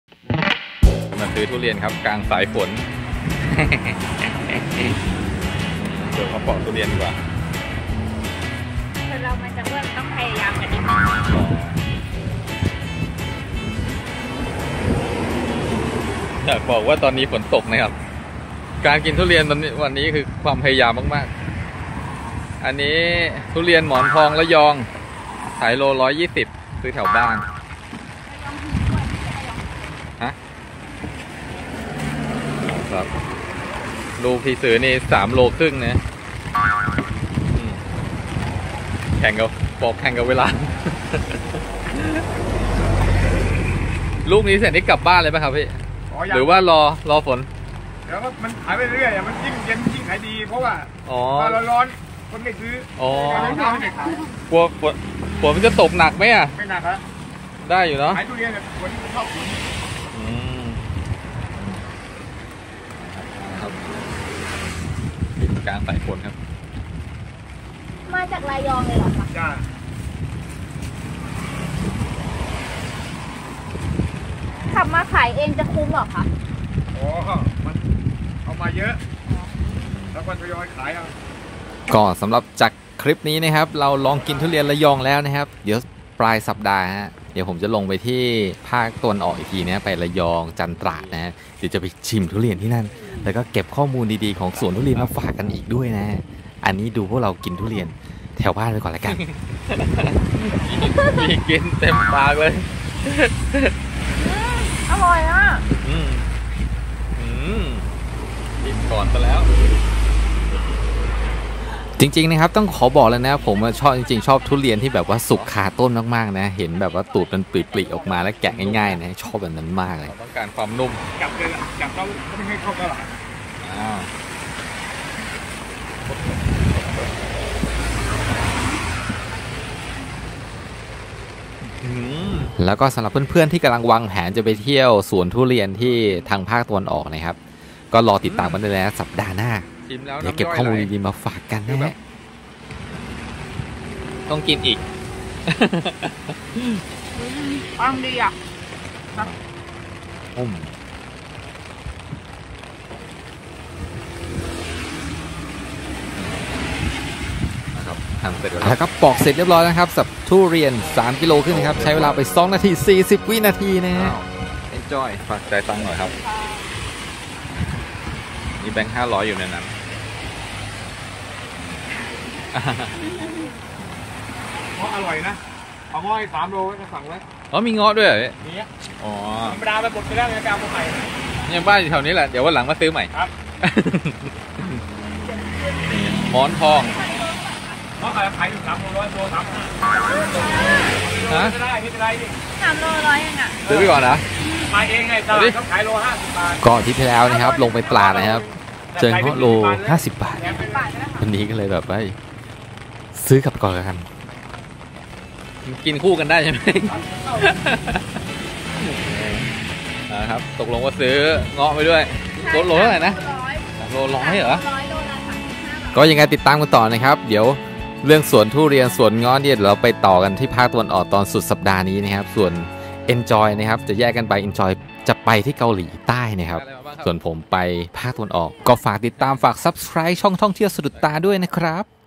พยายามอ้วน ซื้อทุเรียนกลางสายฝน | Vlog's Sadoodta
ตั้งใจมากกับการทุเรียน ขับรถออกมาลุยหาทุเรียน กลางสายฝน นึกว่าจะไม่มีร้านมาตั้งแล้ว แต่ก็ยังมี นี้ละความพยายามของความอ้วน ซื้อทุเรียนกลางสายฝน ...ใครดูคลิปนี้แล้วฝากติดตามด้วยนะ สุดสัปดาห์นี้ผมจะลงไปเที่ยว ระยอง จันทบุรี ตราด จะเก็บข้อมูลเที่ยวสวนทุเรียนมาเพื่อด้วย ส่วนเอนจอย จะไปเที่ยวที่เกาหลีใต้ ถ้ามีที่เวลาเหมาะๆ อาจจะมี Live ให้ชมกันด้วนนะจ๊ะ

ติดตามเราได้ที่ ท่องเที่ยวสะดุดตา
ไลน์ : @sadoodta